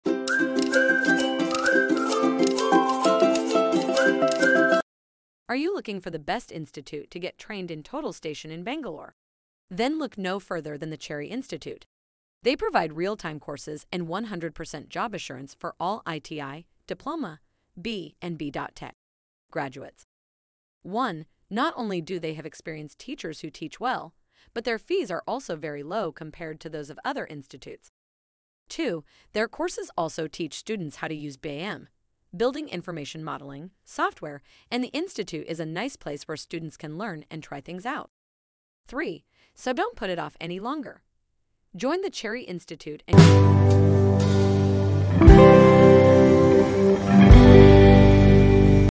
Get TOTAL STATION Training at Bangalore's Lowest Fees - Cherry Institute!
Are you in search of the best Total Station training center in Bangalore? The Cherry Institute is your best choice in that case. All ITI, diploma, BE, and B.Tech. graduates can take real-Time courses from them, and they guarantee them a job at all times.

[1] In addition to employing knowledgeable, experienced professors, they also charge very reasonable tuition fees compared to other educational institutions.

[2] The institute is a nice place for students to learn and explore, and their courses also educate students on how to use BIM (Building Information Modeling) software.

[3] Don't wait any longer; register in Full Station training at the Cherry Institute now!